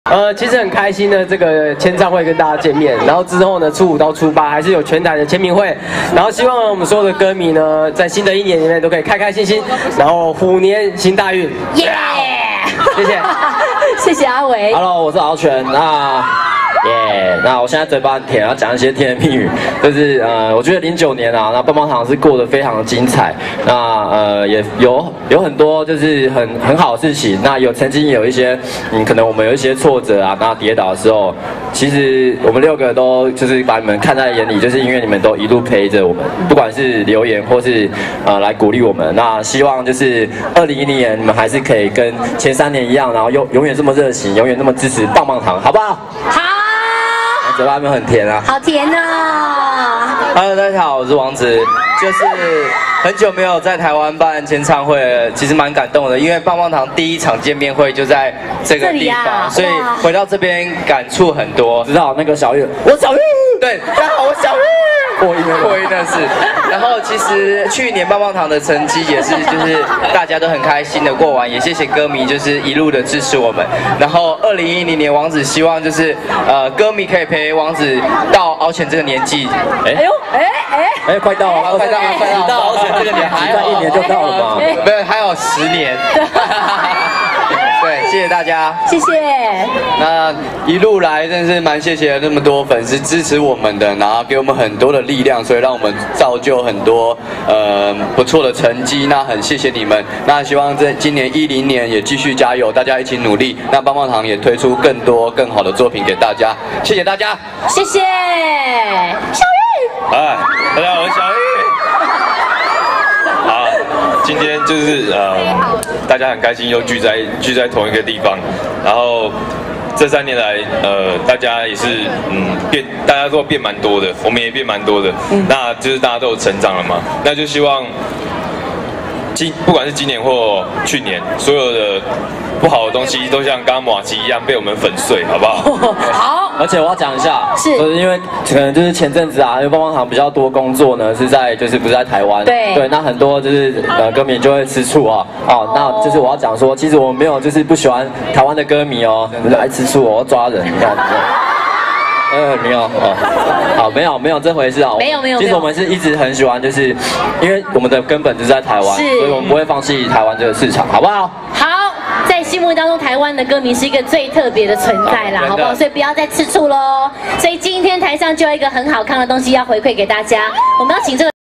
2010-02-06 棒棒堂@台北KMall-4 ~ 六棒分享感想和新年願望 (HD zoom-in版)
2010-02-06 棒棒堂@台北KMall-4 ~ 六棒分享感想和新年願望 (HD zoom-in版)
*** 一整段都好感性的團長敖犬喔 !  2009下半年那些不好的事情已經過去了喇 ~ 明年尾牙就是棒棒堂的天下了 !!!  王子逗趣的說要我們陪他走到敖犬那個年紀 ~ 哈...超可愛的耶 !!! ***